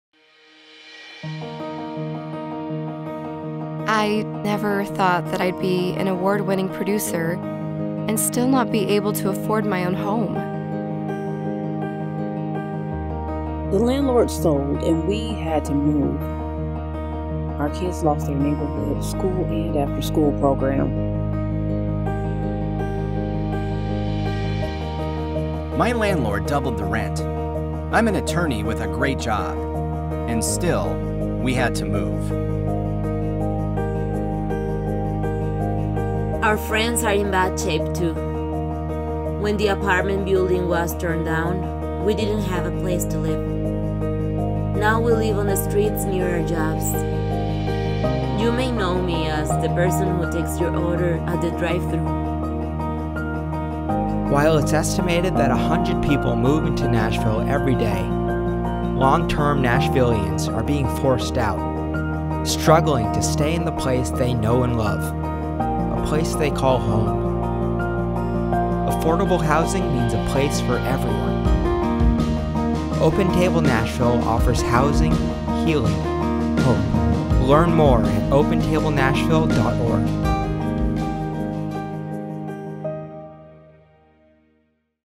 At What Cost?
Created for Open Table Nashville - a non-profit, interfaith community that disrupts cycles of poverty, journeys with the marginalized and provides education about issues of homelessness. Based in Nashville, Tennessee.